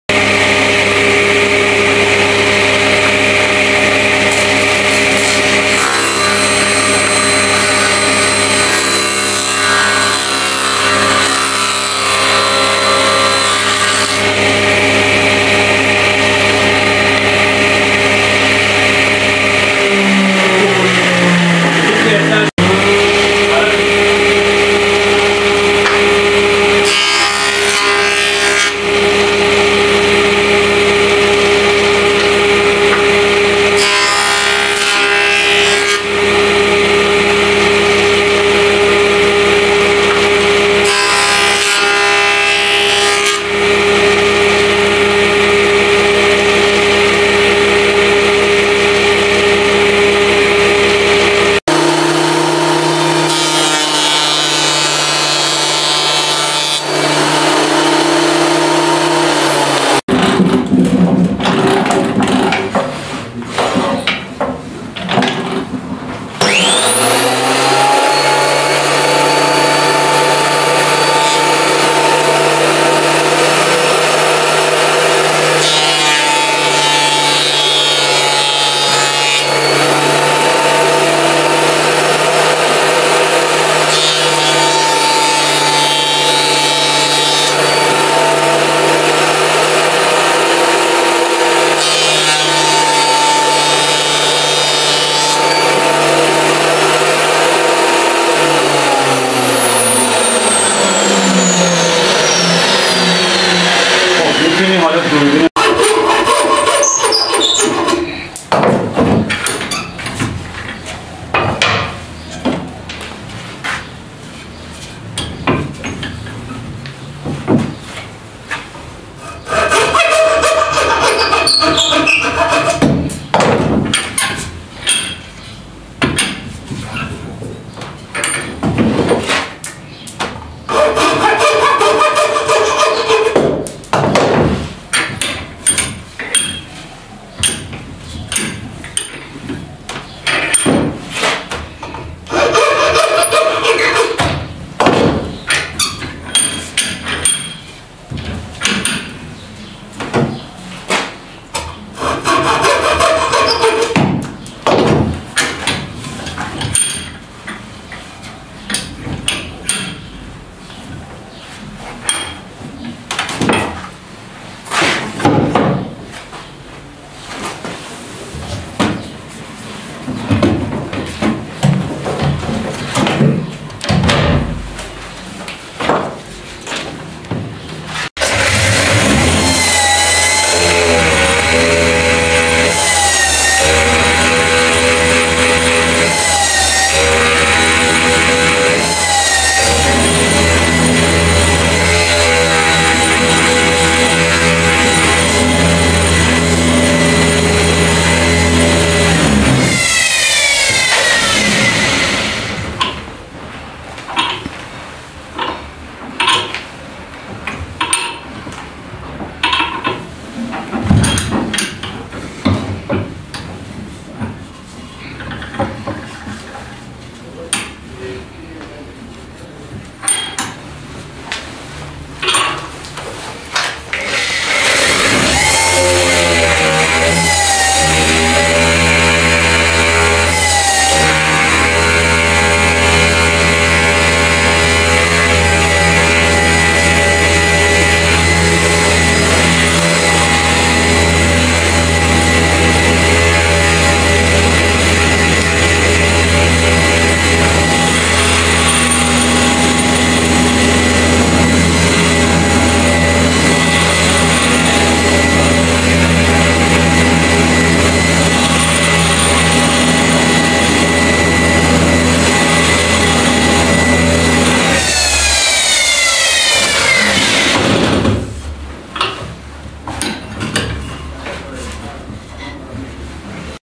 ASMR WOODWORKING! CUTTING, GRINDING, TRIMMING, CHECKING DESIGN!
NONNO ASMR, BY YOUR SIDE!💕